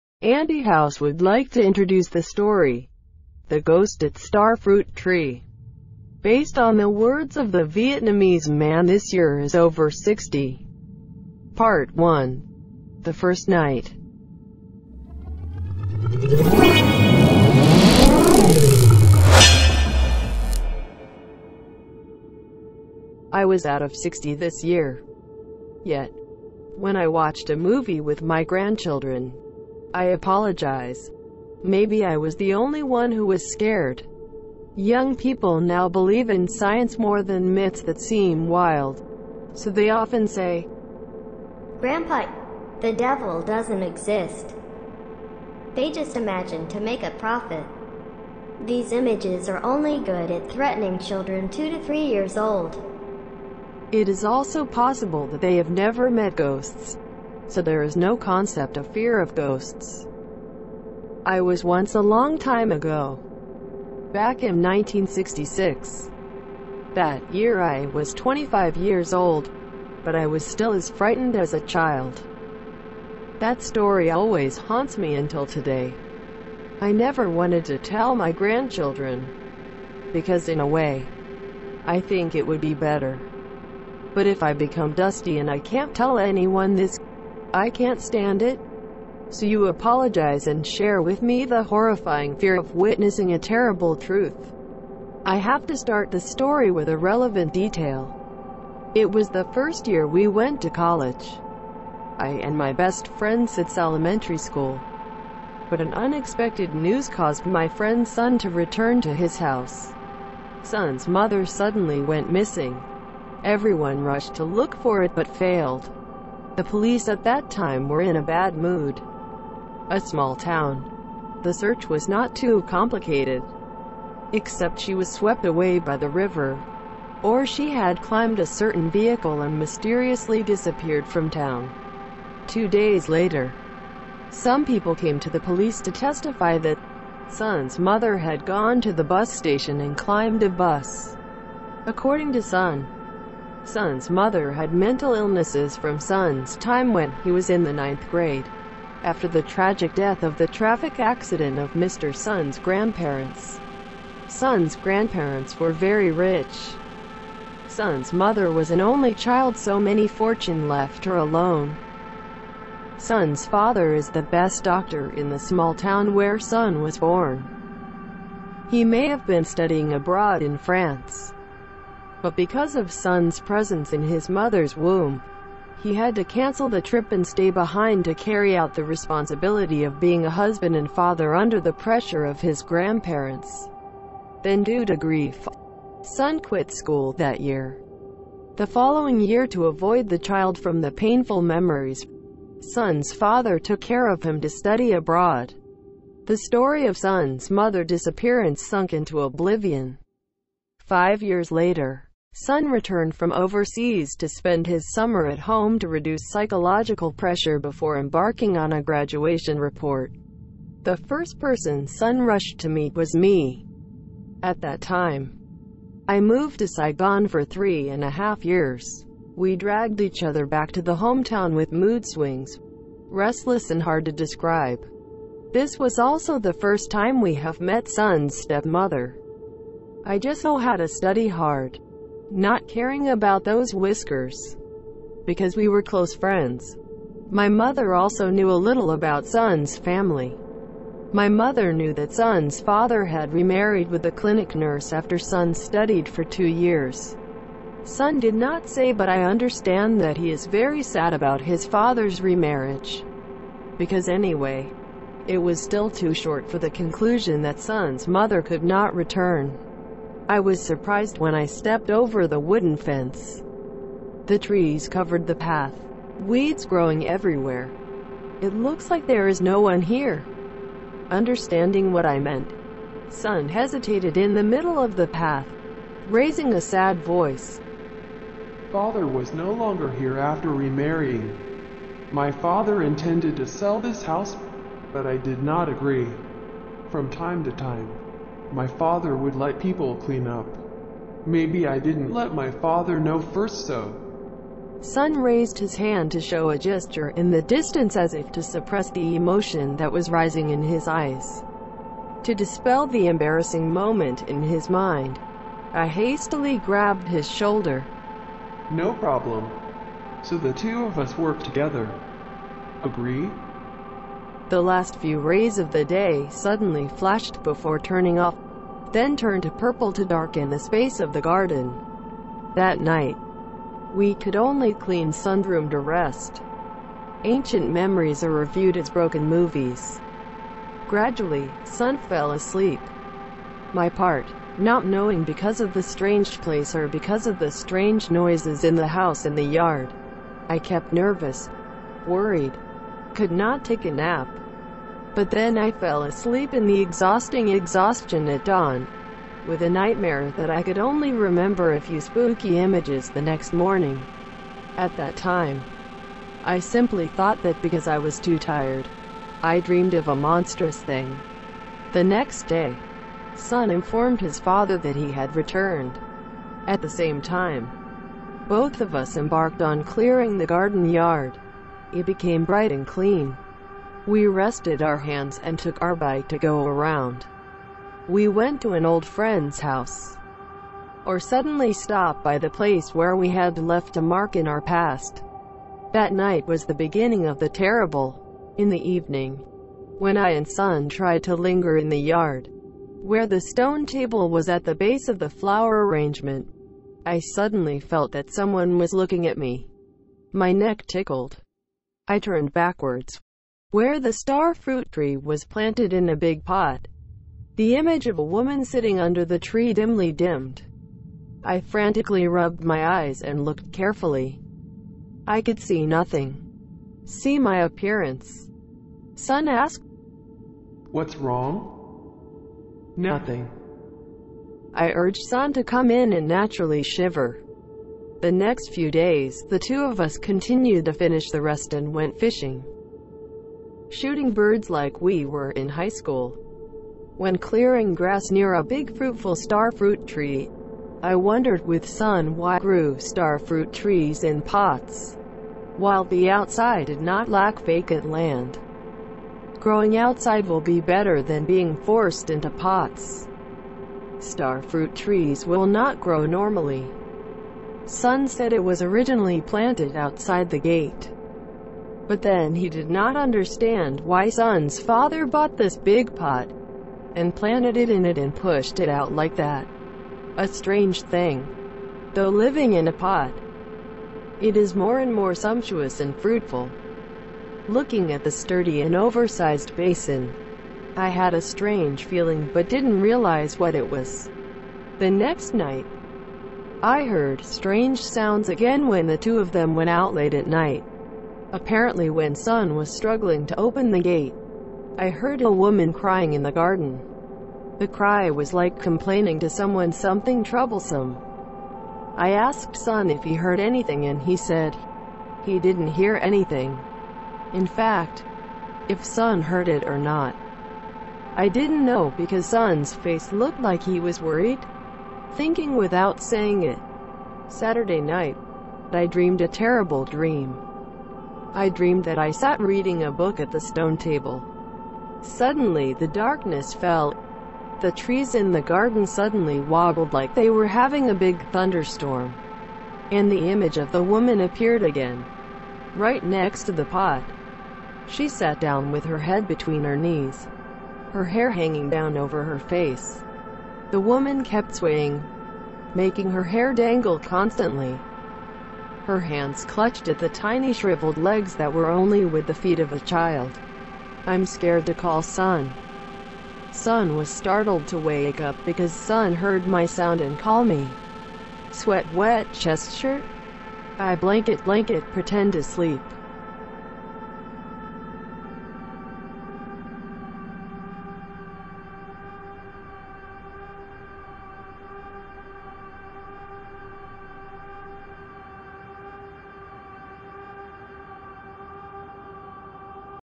The Ghost At Star Fruit Tree | Part 1 - The First Night | AndyHouse
This is a true story told by a man named Duong, who is now in his 60s. He and his close friend, Son, had experienced terrible days when meeting strange things in Son's old house.

AndyHouse - A channel talks about the Vietnamese Scary Stories. They was told by local people and I noted for you.